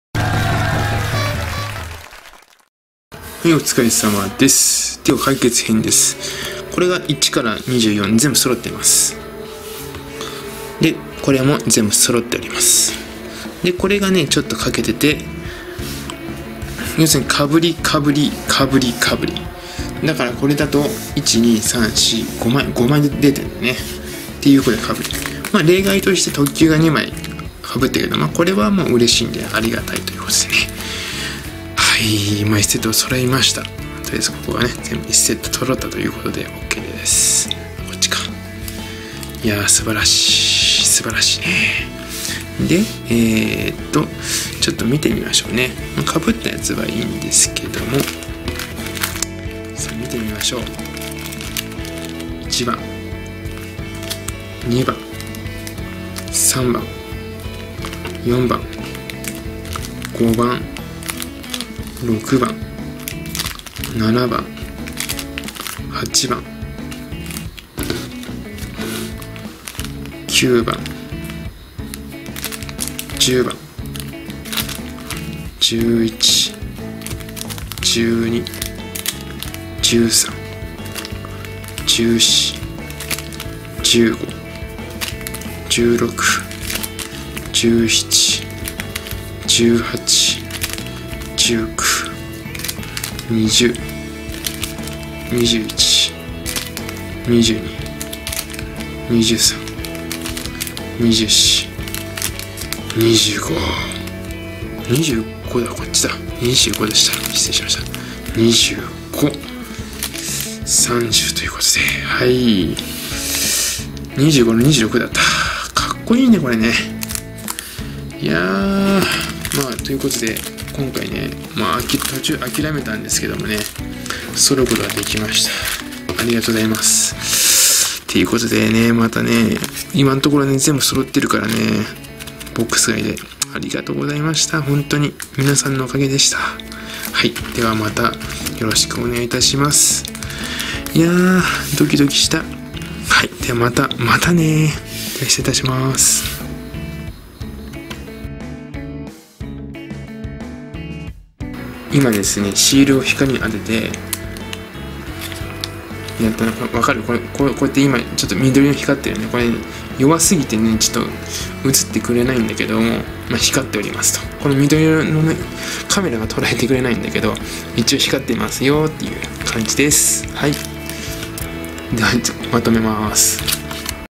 4BOX開封結果発表！呪術廻戦ディフォルメシールウエハース第1弾！！ はたしてボックス開封はなんだったのか！！？？
応援ありがとうございました！
結果がどうあれ
やっぱりボックス開封は楽しいです！
皆とワイワイ開封するのが幸せです！
ただコンプするだけならメルカリのほうがコスパはいいですが
やっぱりﾄﾞｷﾄﾞｷ楽しみたいならボックス開封です！！
観てくれてありがとうございました！
ではまたあああああ！！

もしよかったら他の開封動画も観てください